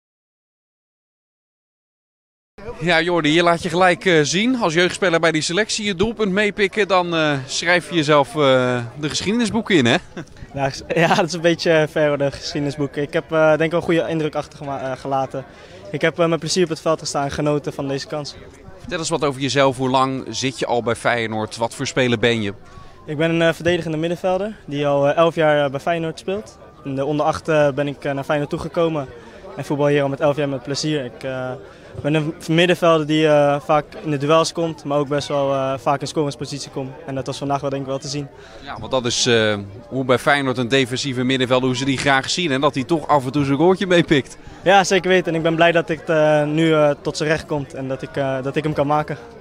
Jordy Wehrmann: 'Genoten van deze kans'
Feyenoord speelde tijdens het oefenduel tegen FC Lisse (5-0) met twee rechtsbacks: Bart Nieuwkoop en Kevin Diks. Een van hen moet Rick Karsdorp, die een transfer maakte naar de Italiaanse topclub AS Roma, op de rechtsbackpositie gaan opvolgen.Nieuwkoop heeft bij Feyenoord nummer 2 gekregen, het oude rugnummer van Karsdorp. De verdediger uit Zeeland focust zich volledig op de rechtsbackpositie: &quot;Daar liggen voor mij de meeste kansen om me verder te ontwikkelen.&quot;Samen met Kevin Diks, op huurbasis overgekomen van Fiorentina, moet Nieuwkoop gaan uitmaken wie de nieuwe rechtsback van Feyenoord wordt. &quot;Ik ken Diks goed en heb veel met hem gespeeld in de jeugdteams van Nederland. Hij is een goede vriend van me. Ik ben blij dat hij er is. We gaan samen de concurrentiestrijd aan. Onze vriendschap gaat daardoor niet verloren.&quot;JeugdDoor het ontbreken van een aantal basisspelers gaf Feyenoord-coach Giovanni van Bronckhorst tijdens het gewonnen oefenduel tegen FC Lisse een paar jeugdspelers de kans. Dat gold onder andere voor Jordy Wehrmann, die ook nog eens trefzeker was voor de Rotterdammers.&quot;Ik heb denk ik een goede indruk achter gelaten. Vandaag stond ik met plezier op het veld en heb ik genoten van deze kans&quot;, zegt Wehrmann, een verdedigende middenvelder. &quot;Ik speel al elf jaar in de jeugdopleiding van Feyenoord. Ik ben een middenvelder die vaak in de duels gaat, maar ook in scoringspositie komt. Dat was vandaag, denk ik, wel te zien.&quot;